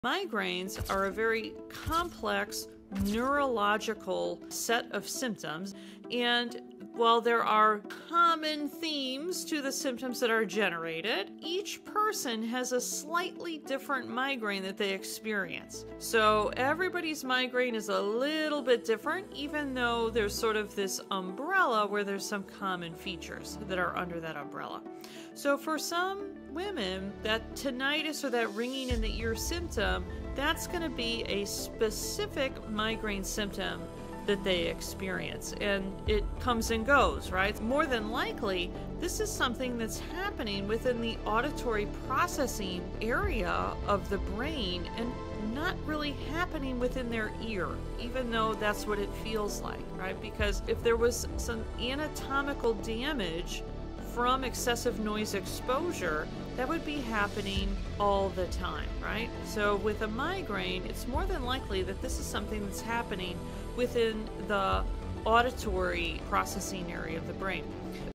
Every migraine has its own “fingerprint” 🧐
🧠 Every migraine has its own “fingerprint.” While some women experience nausea or light sensitivity, others experience tinnitus, that high-pitched ringing in the ears, as part of their migraine cycle.

Here’s the key insight 👇
If the ringing comes and goes, it’s probably not permanent ear damage. It’s more likely happening in the auditory processing center of your brain, not your ear itself. Understanding this difference changes everything about how you approach healing. Click the pinned video for the full episode!